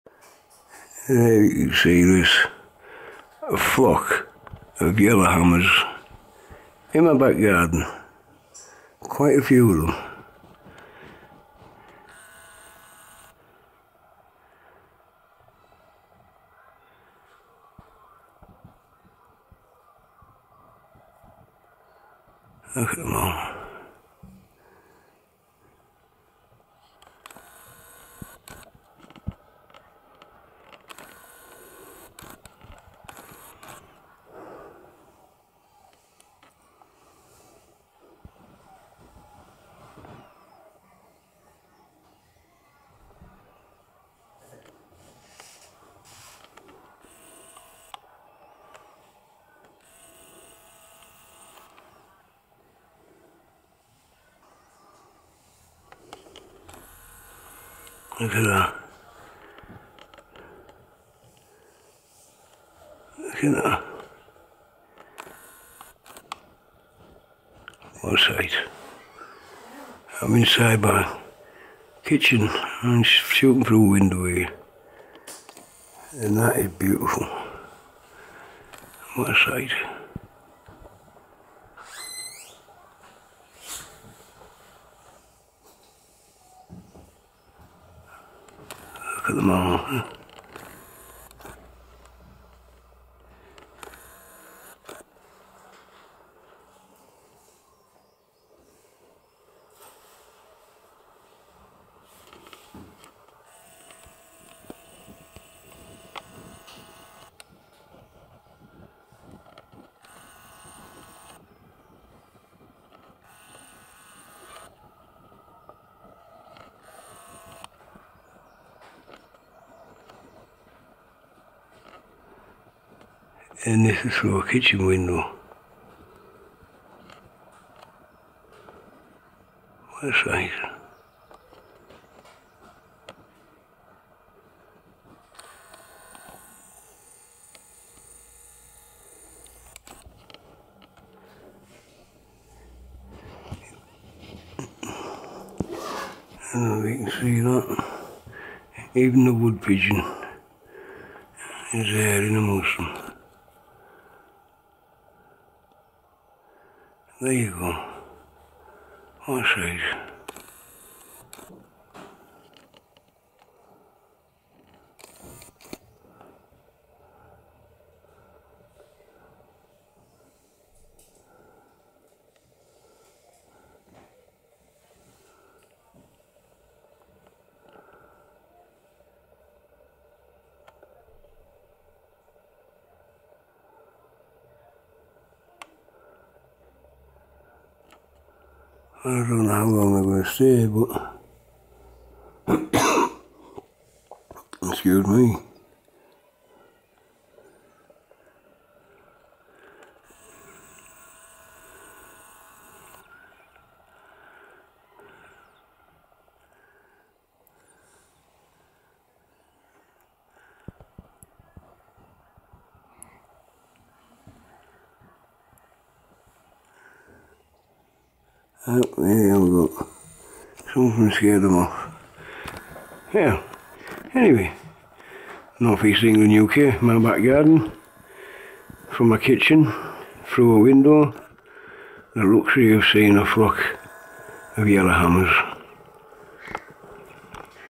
BIRDS ARE STILL THRIVING..YELLOW HAMMER
SO BLESSED TO CAPTURE A FLOCK OF YELLOW HAMMER IN MY BACK GARDEN, shot through double glazed windows and rain drops,  from inside my kitchen,  :) ... first i thought they had stumbled on bird seed from my feeder blown off in the wind,.. then i noticed they where feeding off the water droplets on the blades of grass ...has to be more than 30?... biggest flock of yellowhammers i,ve ever seen ...:o how cute ...northeast england uk